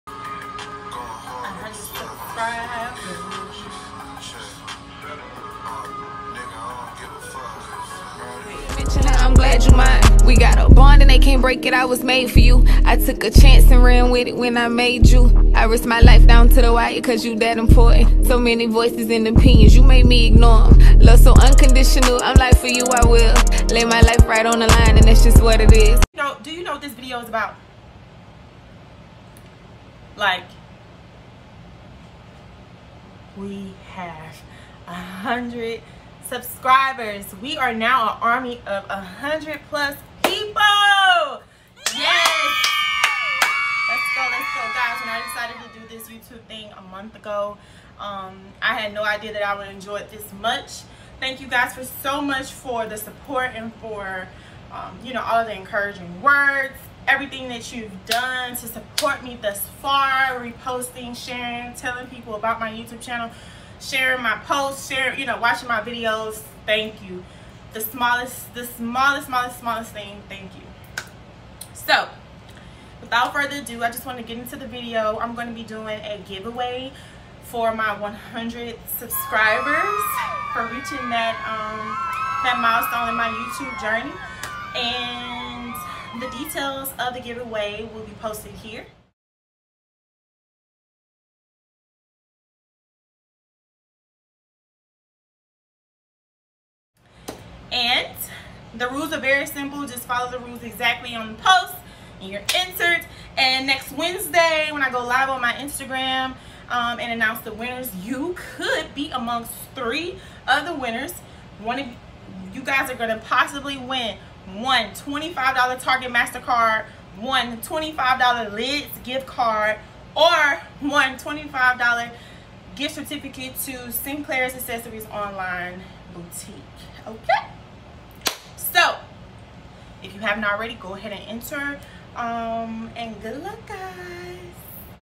100 SUBSCRIBERS! | GIVEAWAY [CLOSED] | THEREALBELLAONE
Thank you for sharing in this journey with me. The support thus far has been amazing and encourages me to keep going. In 30 days we have become a 100 man army! I hope you have enjoyed me as much as I have enjoyed you!

As a token of my appreciation I want to show love and bless you guys and hopefully encourage others to join us. As I always say, you must BE a blessing to be BE BLESSED!

Follow the rules in the post above! You must follow ALL rules, this is the only way to enter. 3️⃣ lucky Winners will be announced via my IG Live next Wednesday 7/22 @7pm CST